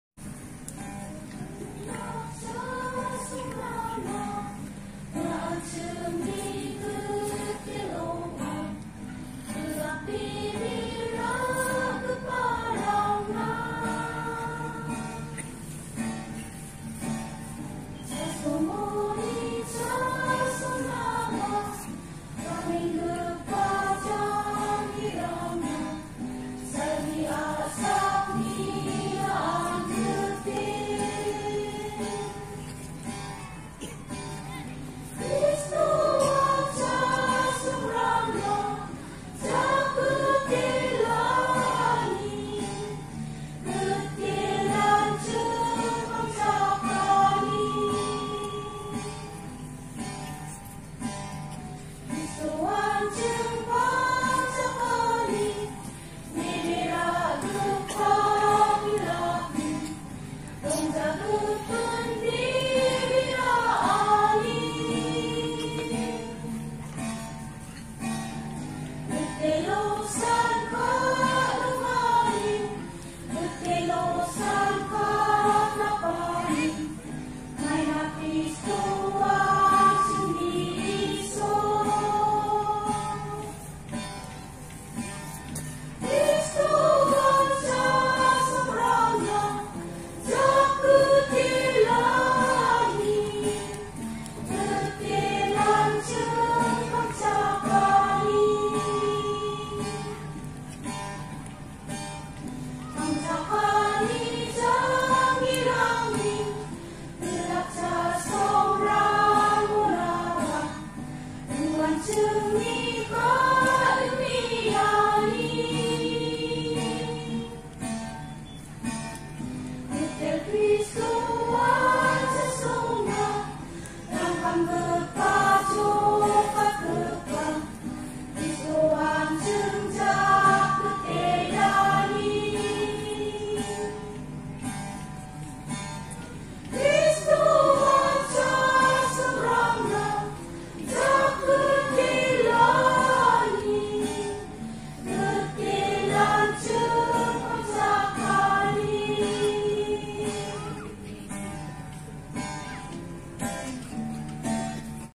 Kristoan chasongrangna jakgitelani
Angni video rangko nisoanina sakantikon angni gisik ka.tong gimikchi mitela.

Welcome song Rajapara 25 silver jubilee